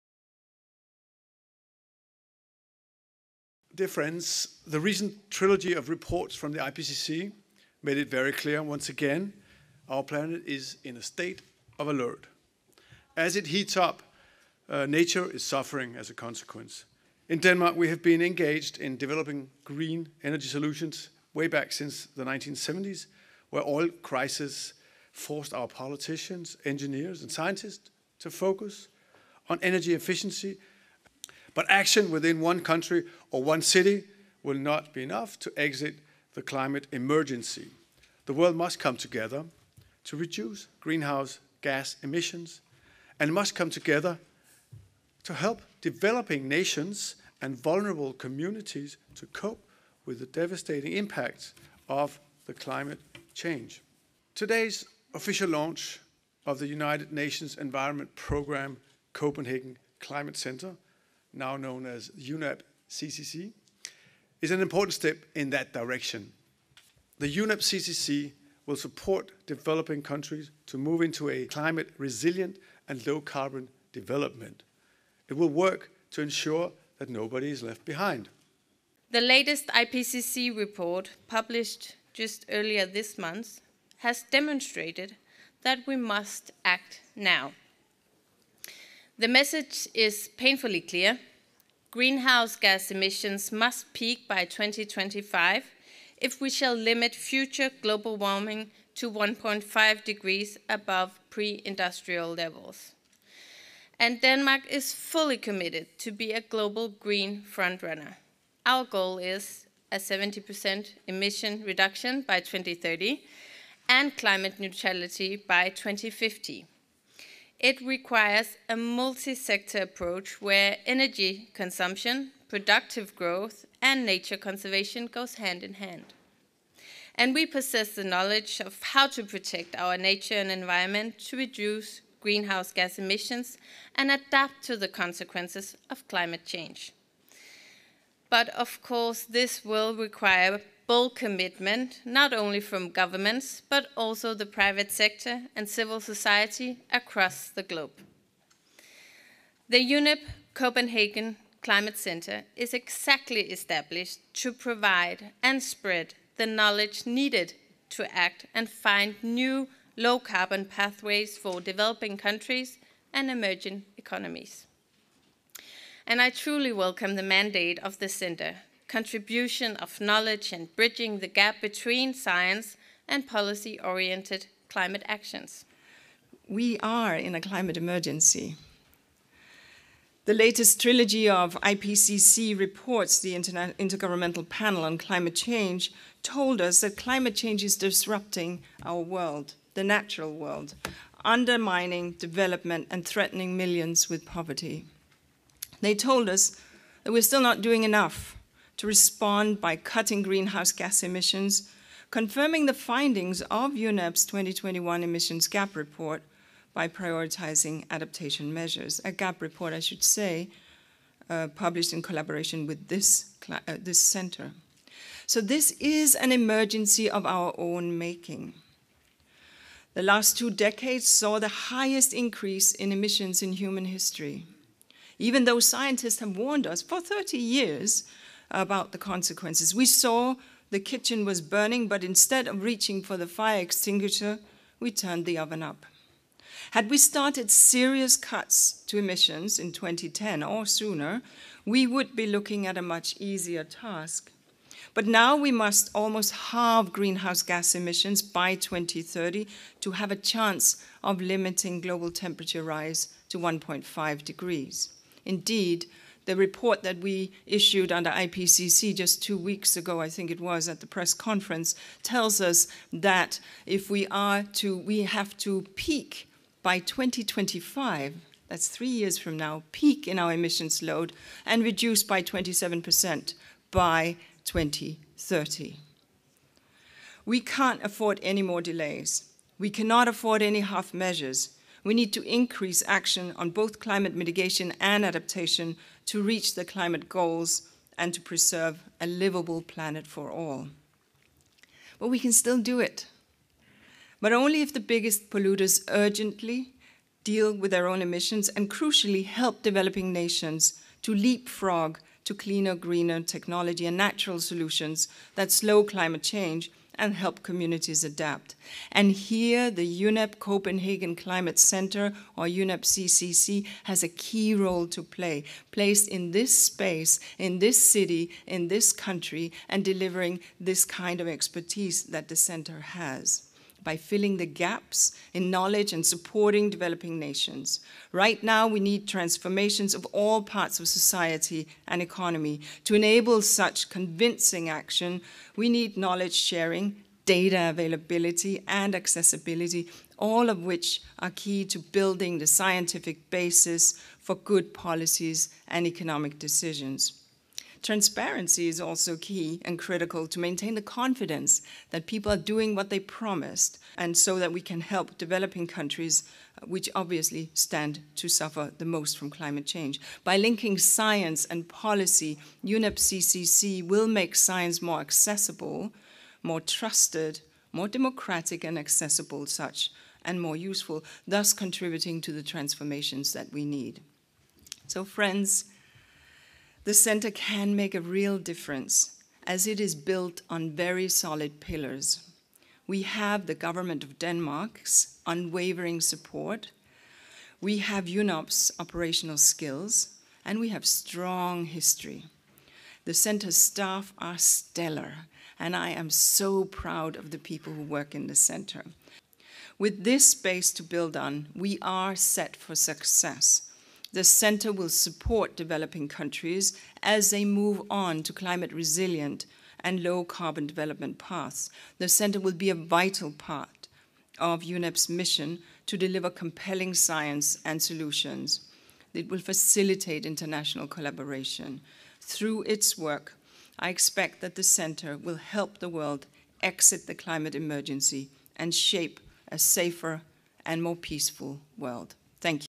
UNEP CCC official opening 2022
A short video with samples from some of the speeches at the official opening of UNEP Copenhagen Climate Centre April 26th 2022.

Speakers:
HRH the Crown Prince of Denmark
Lea Wermelin, Minister of Environment of Denmark
Inger Andersen, UNEP Executive Director and UN Under-Secretary General